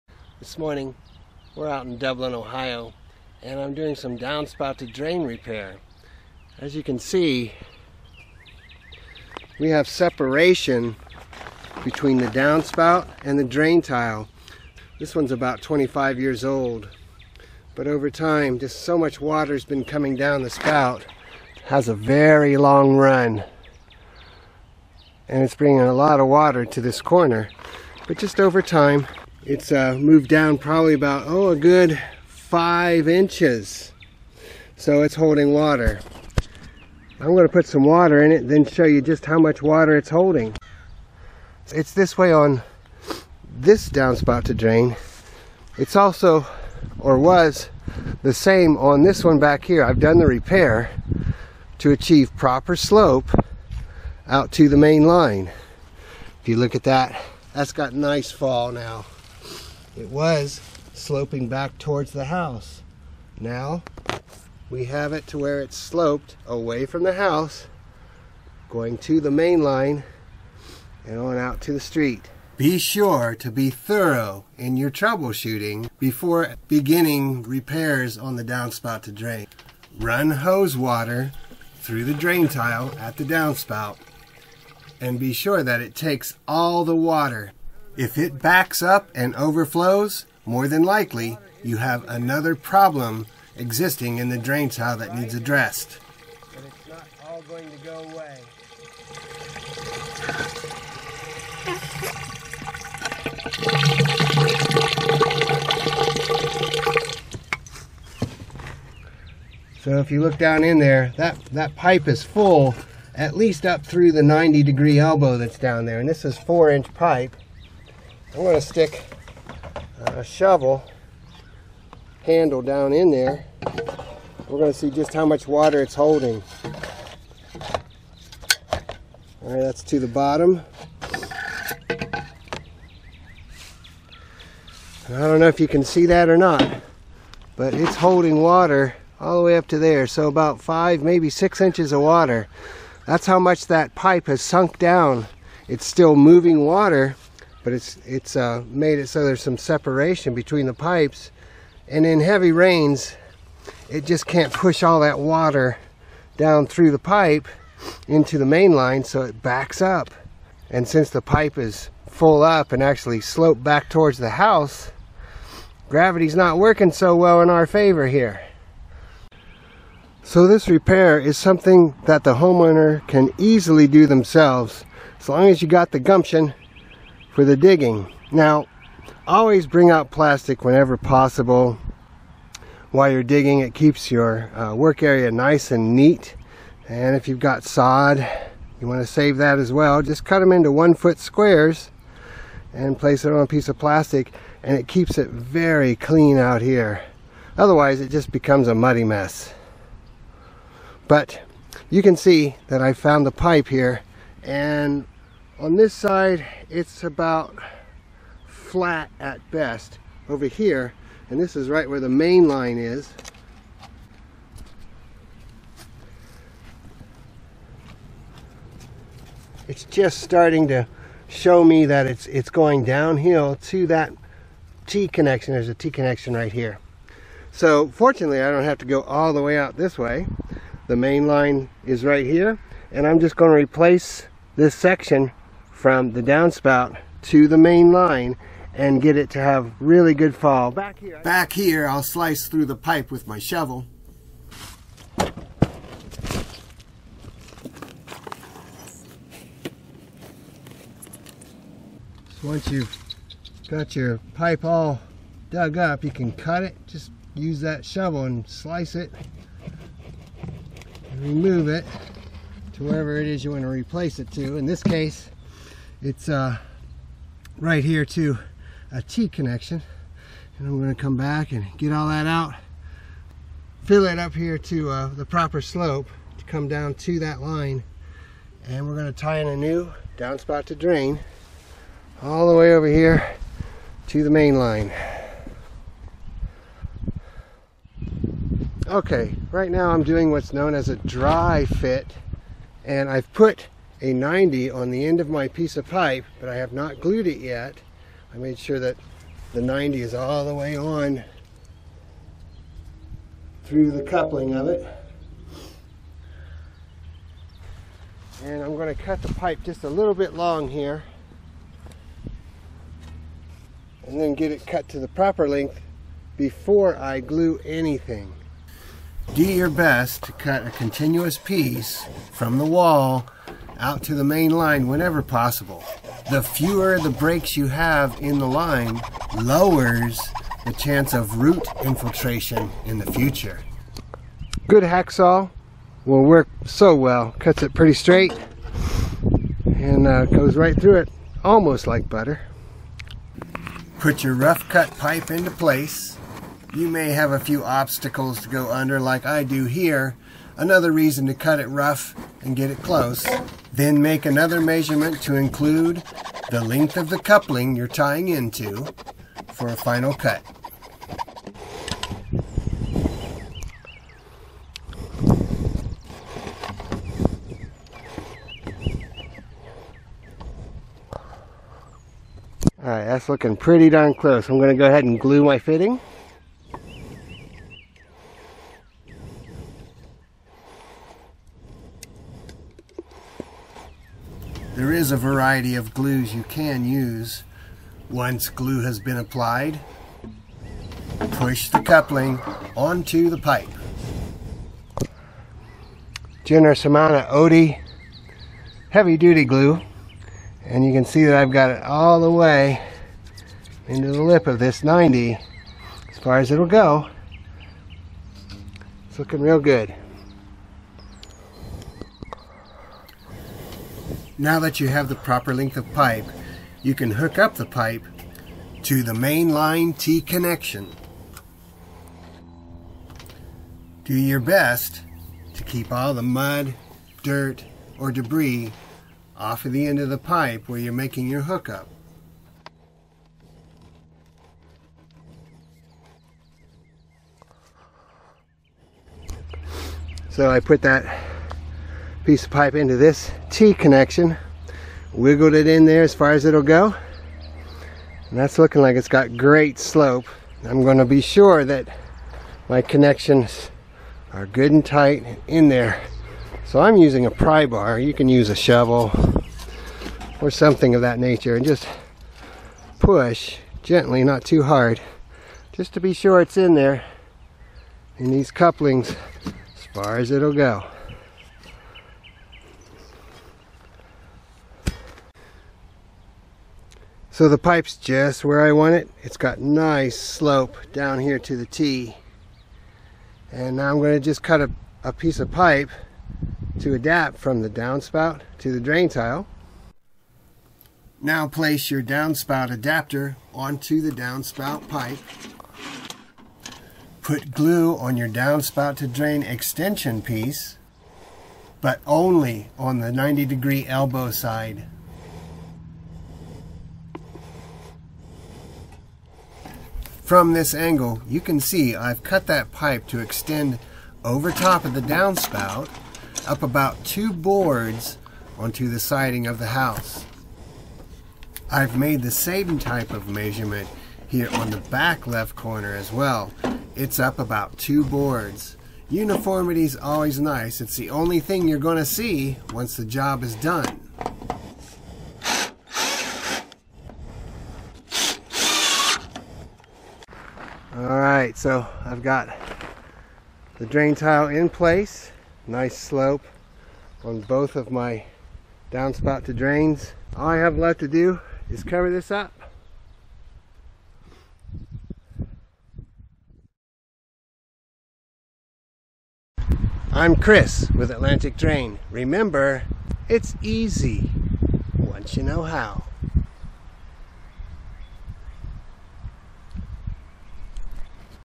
Big Gap Between Downspout and Drain? Howto repair PVC Pipe holding water at the downspout
This video shows why separation has occurred & How to make proper repairs to 4" PVC Pipe where the downspout meets the drain. Aka downspout to drain repair. Why has this occurred at my downspouts PVC Drains? Downspout drain slope issues cause wet basement wall -basement waterproofing solutions